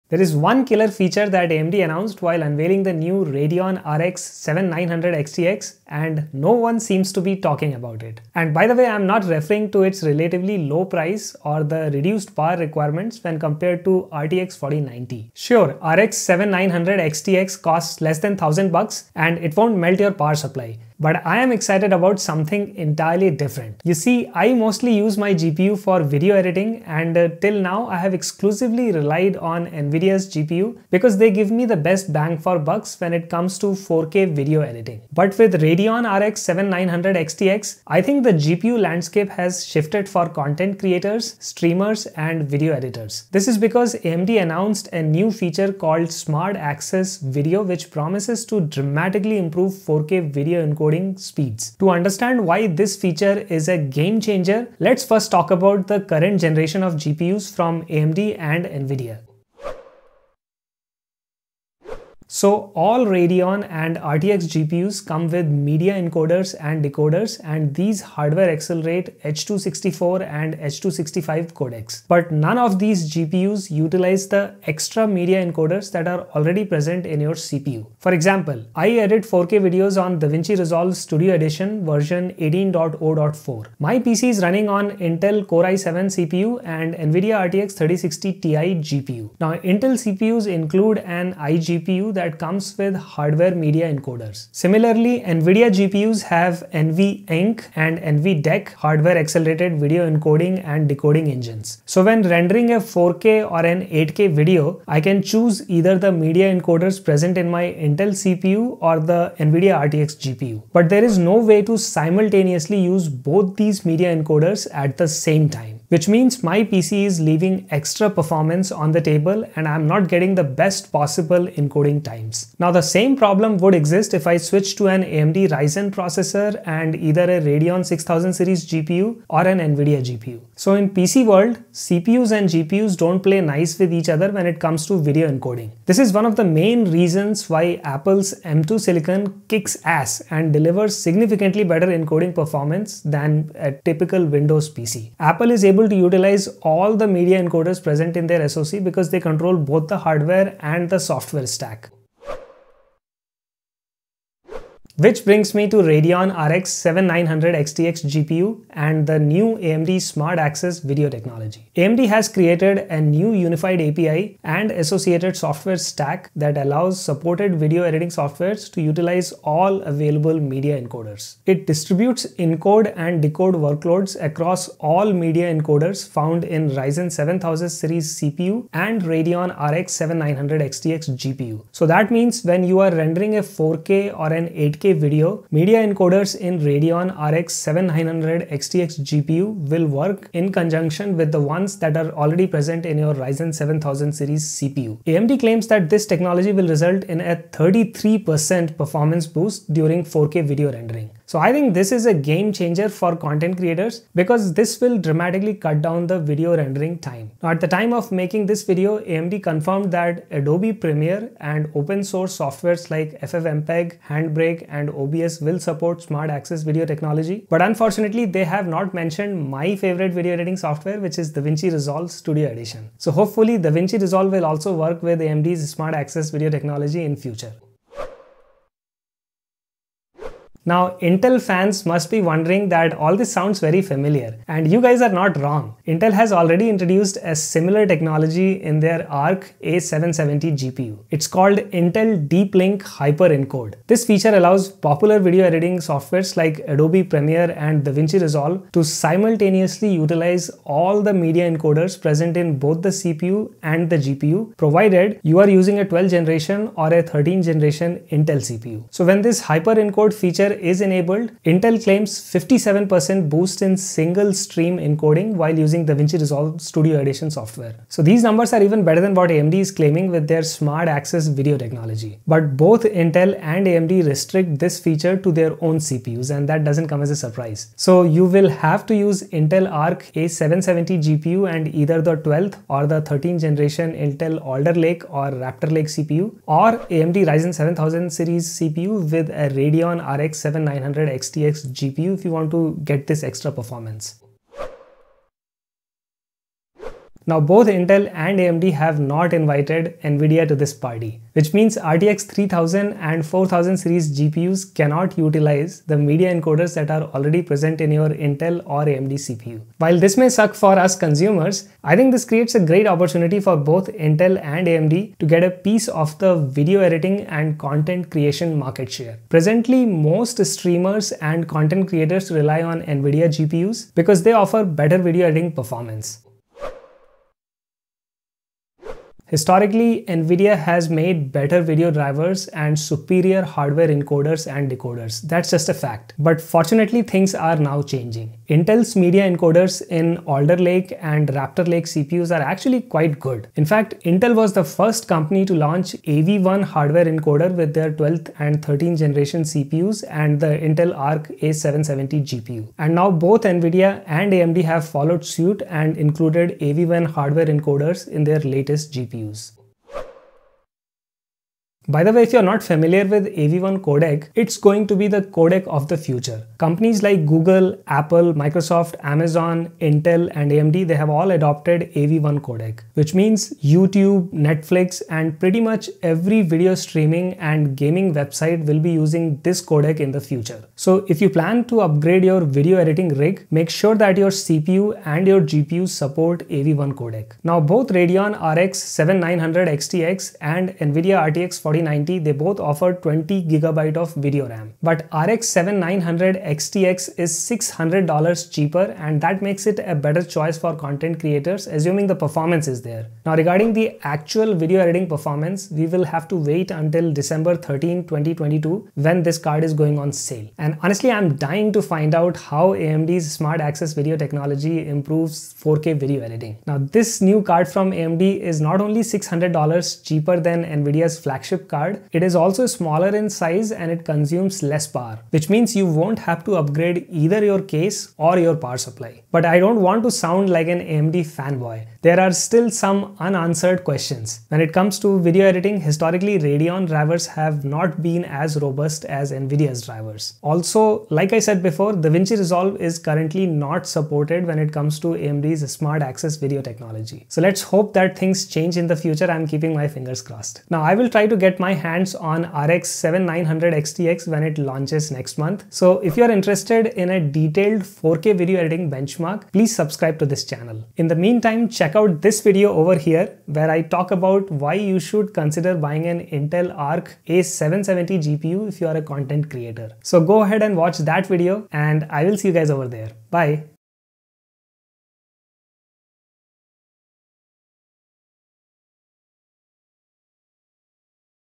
AMD RX 7900 XTX vs Nvidia RTX 4090: 4K Video Editing!
💰 Get Unlimited 5G Data @ $15/Month 💰

Is AMD Radeon RX 7900 XTX good for content creators, streamers and video editor? AMD announced a new feature called SmartAccess Video which promises to dramatically improve 4K video encoding speeds. At the time of making this video, AMD confirmed that Adobe premier and open source softwares like ffmpeg, handbrake and OBS will support Smart Access Video technology.

My Favorite Video Editing GPUs

🙏 If you found this video useful, please consider supporting this channel